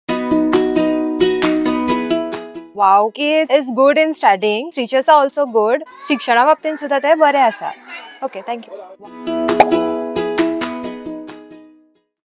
Parents Trust Us! See What Makes WowKids Preschool Special - Aditya & Aruna
At WowKids Preschool, Aditya & Aruna share their journey and how WowKids has provided a nurturing environment for their child's growth. With a focus on creativity, learning, and fun, WowKids Preschool helps children thrive every day.